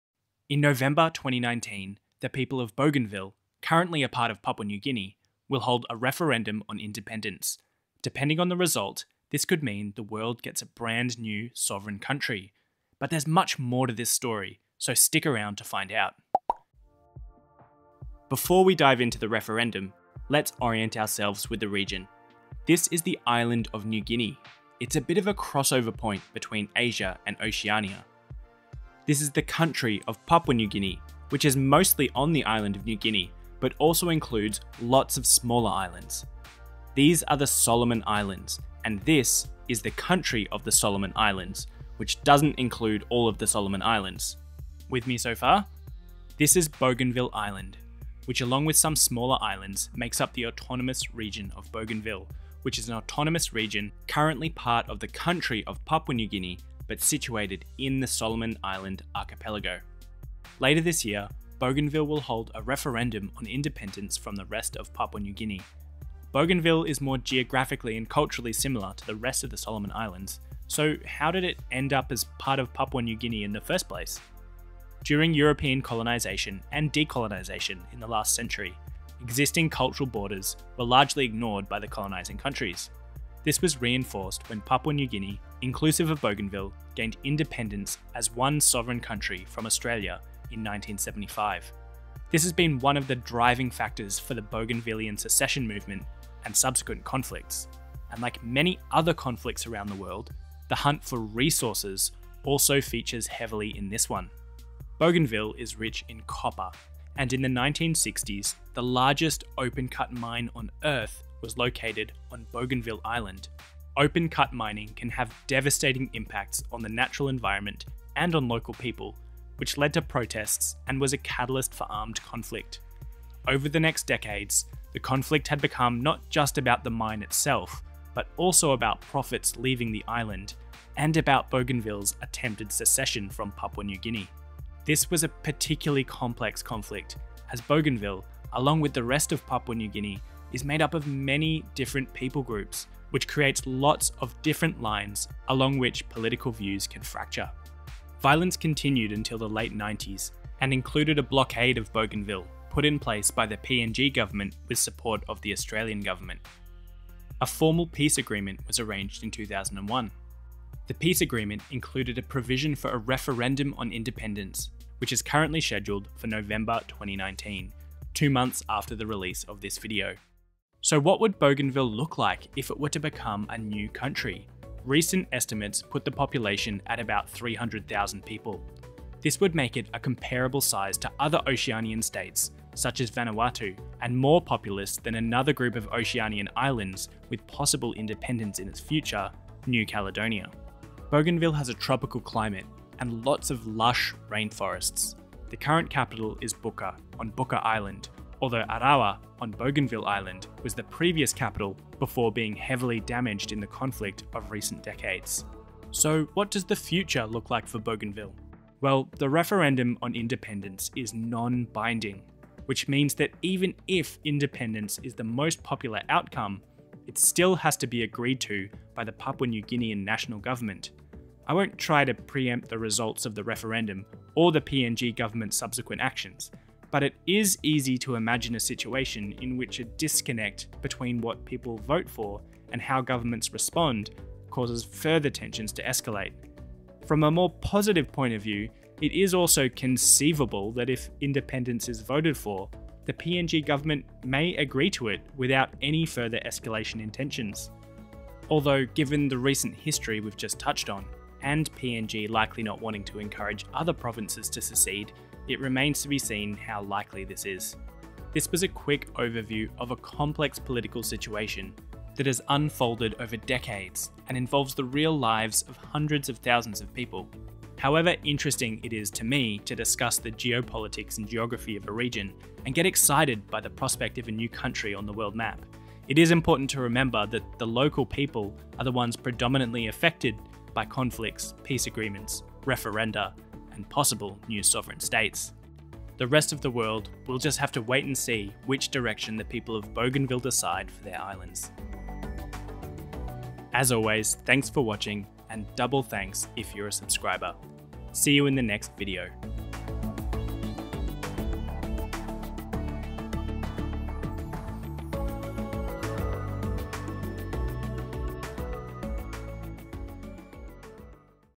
Will Bougainville Be A New Country? - Bougainville Independence Referendum 2019
Footage used in this video courtesy of Skyborne Visions LLC, check them out!

SUPPORT TTK:
MERCH 🎽:

The Autonomous Region of Bougainville is a group of islands currently a part of Papua New Guinea.
Bougainville will hold an independence referendum to decide wether to become a seperate sovereign state from Papua New Guinea in November 2019.
However the Bougainville Independence Referendum will be non-binding, so it is unclear what will happen if independence from Papua New Guinea is voted for.
Find out a brief overview of the Autonomous Region of Bougainville and why the AROB is holding a referendum on independence from Papua New Guinea.

Credits:
Footage courtesy of Skyborne Visions (used with permission)
Image of Earth courtesy of NASA